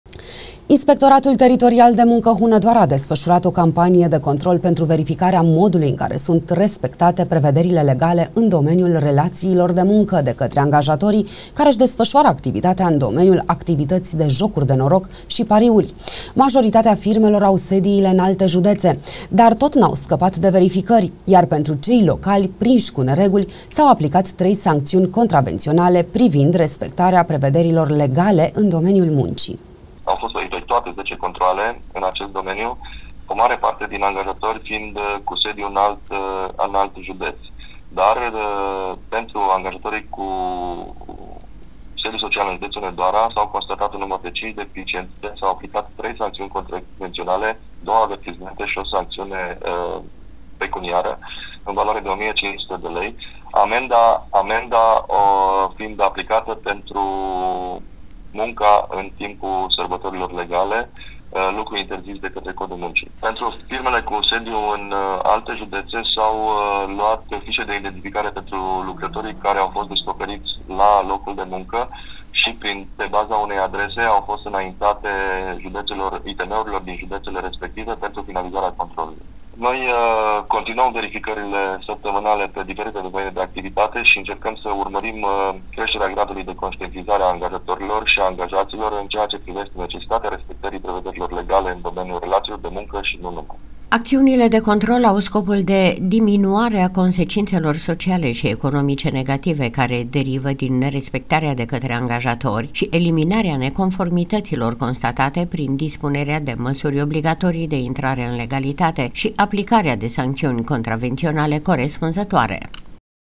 ACTIVITĂȚILE DE JOCURI DE NOROC ȘI PARIURI, LA CONTROL
INSPECTORII I.T.M. HUNEDOARA AU APLICAT TREI SANCȚIUNI CONTRAVENȚIONALE

Inspectoratul Teritorial de Muncă Hunedoara a desfășurat o campanie de control pentru verificarea modului în care sunt respectate prevederile legale în domeniul relațiilor de muncă, de către angajatorii care își desfășoară activitatea în domeniul „Activități de jocuri de noroc și pariuri. Majoritatea firmelor au sediile în alte județe, dar tot n-au scăpat de verificări, iar pentru cei locali, prinși cu nereguli, s-au aplicat 3 sancțiuni contravenționale respectarea prevederilor legale în domeniul muncii.